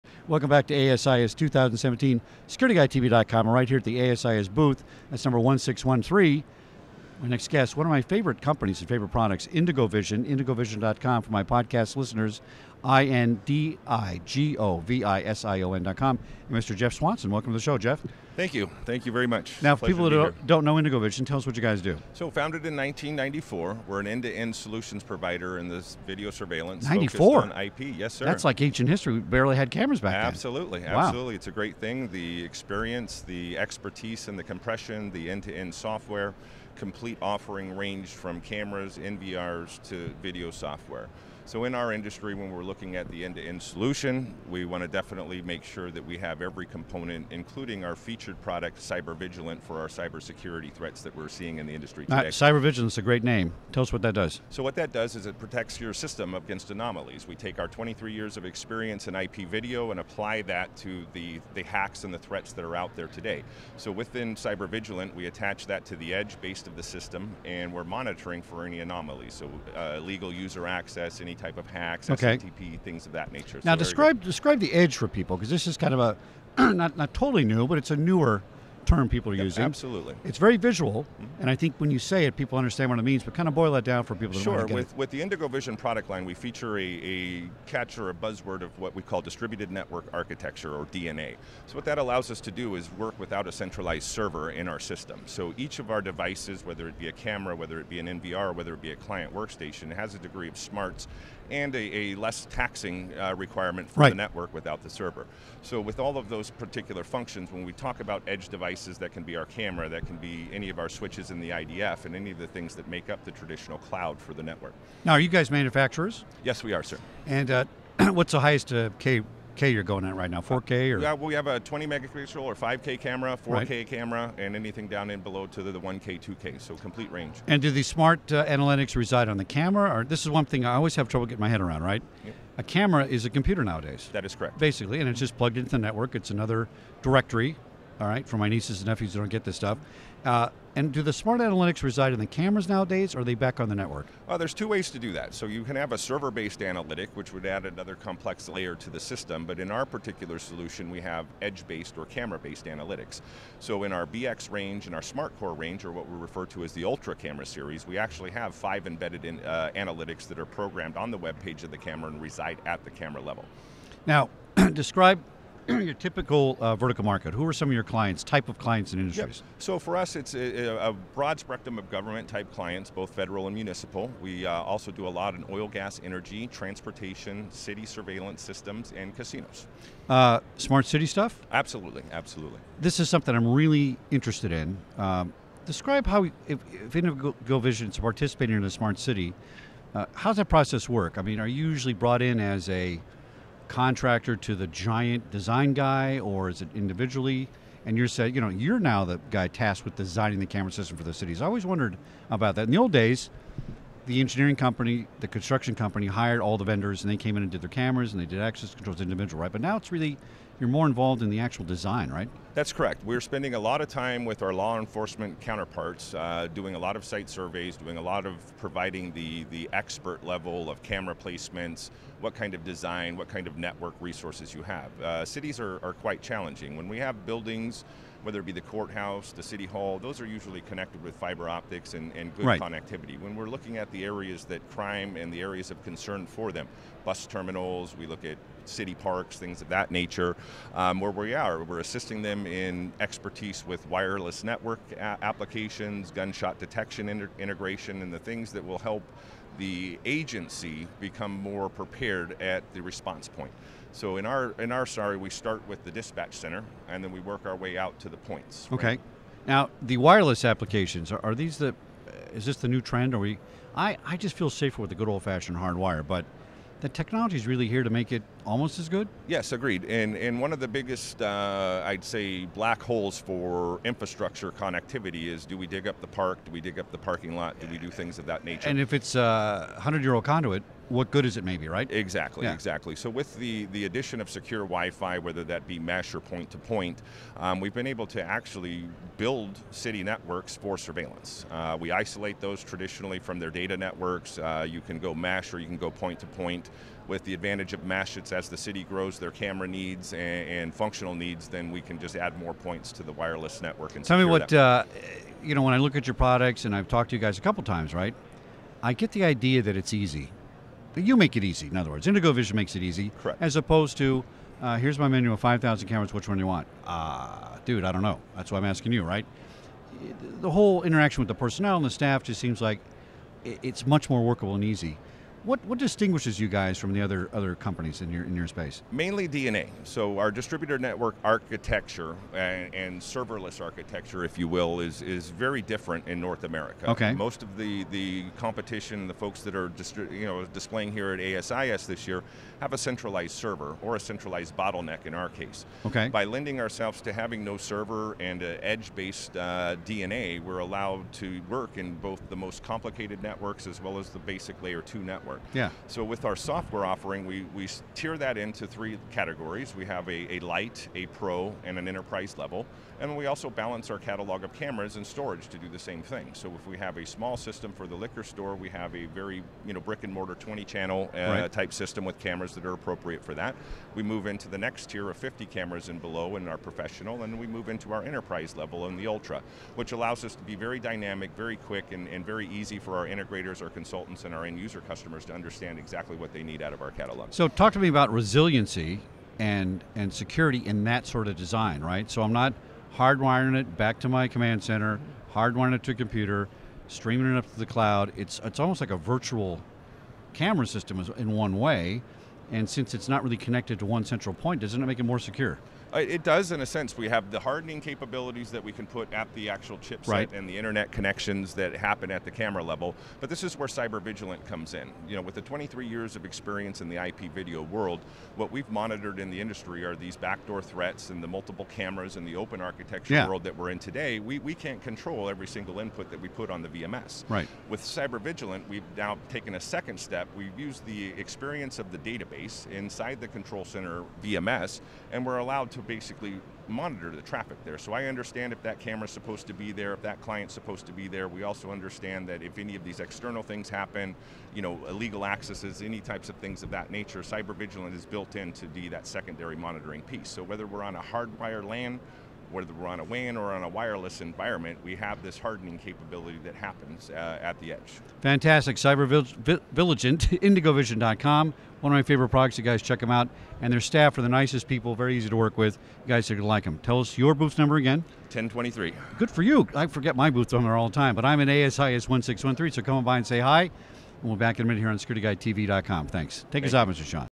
IndigoVision’s Jeff Swanson meets with Chuck Harold at ASIS17
Interviewed at ASIS International Dallas 2017, Jeff Swanson talks to Chuck Harold at Security Guy Radio about IndigoVision’s latest in security solutions!